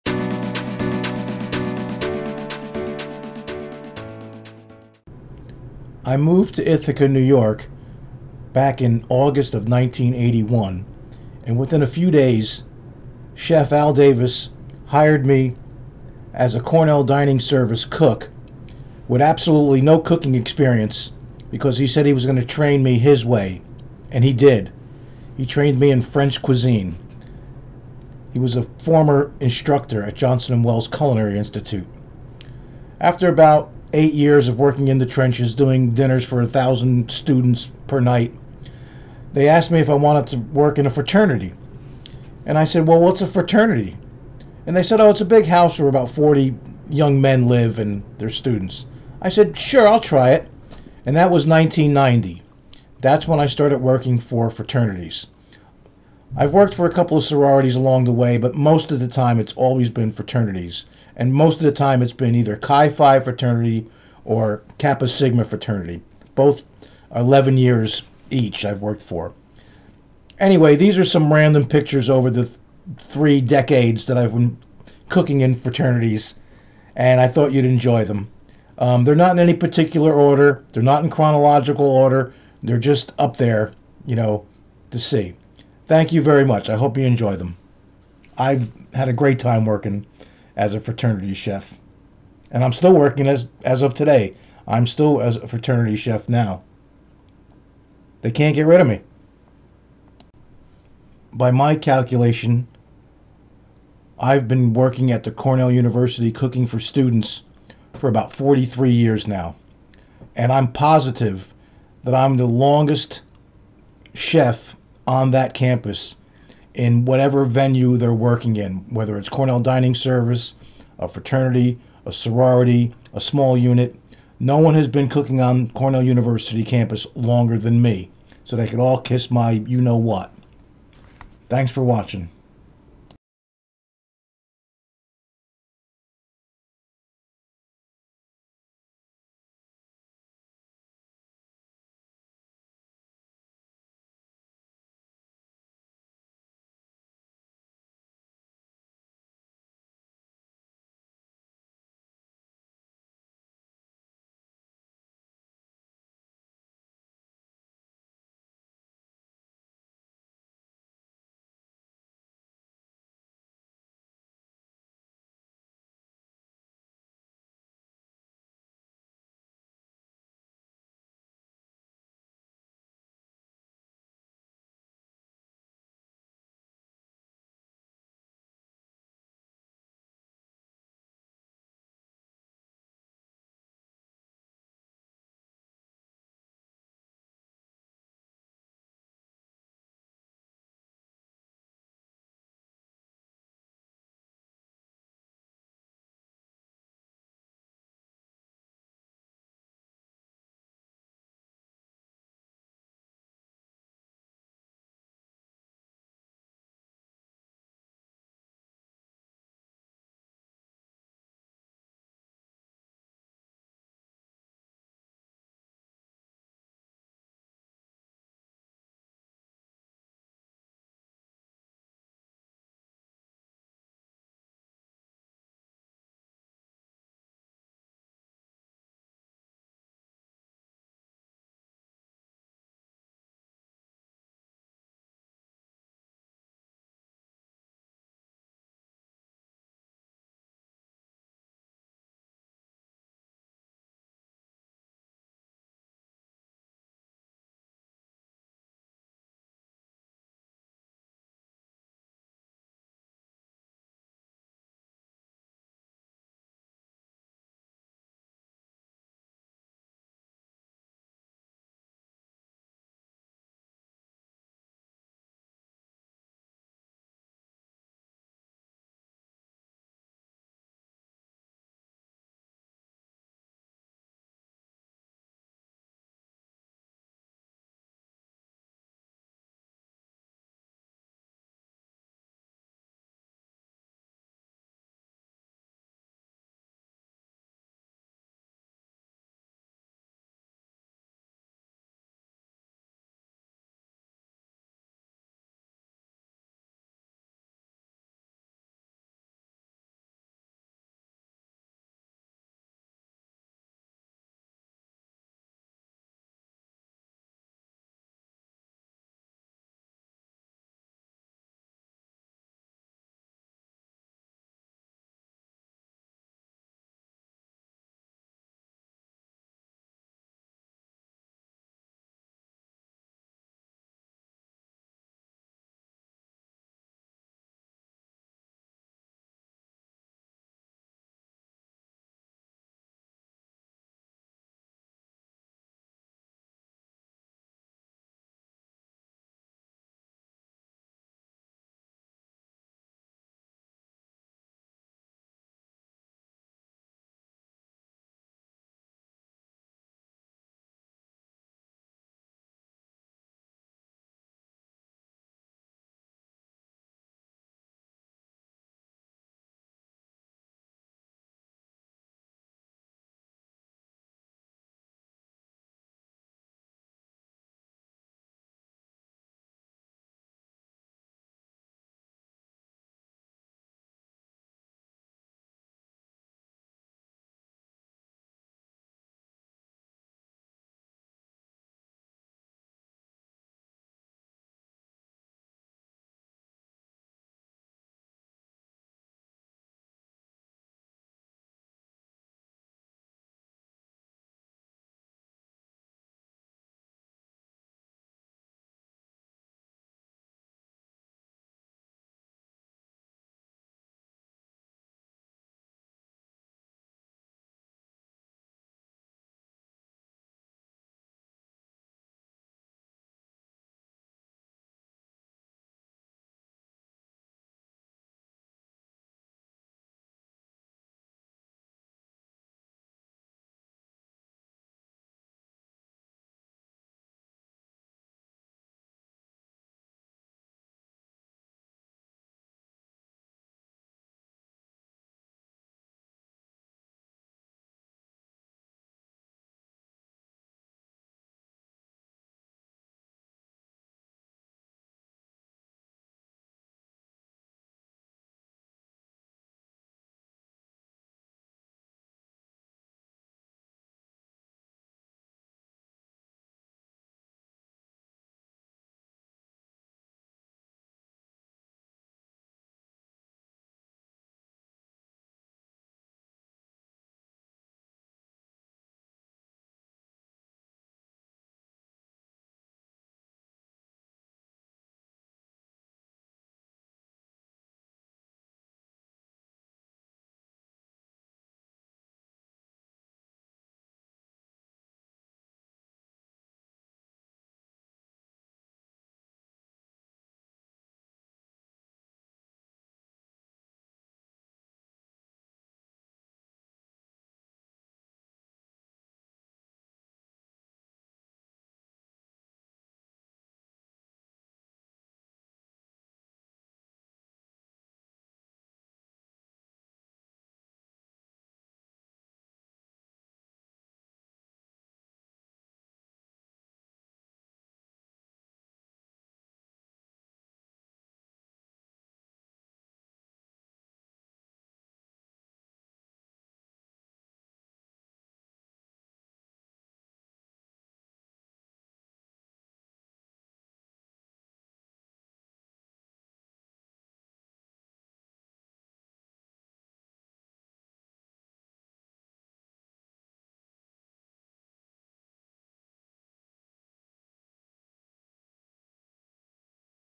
FRATERNITY CHEF FOR 34 YEARS AND COUNTING
I've been cooking for Cornell University students in Ithaca NY since 1981. Since 1990, I've cooked in fraternities and sororities, and I have no plans to retire. I've cooked for Cornell students longer than any other current chef/cook on the campus. Chef Al Davis gave me a chance. I had zero experience cooking, and he taught me French Cuisine. I think about him ever day at work, and I'm so grateful to him for teaching me.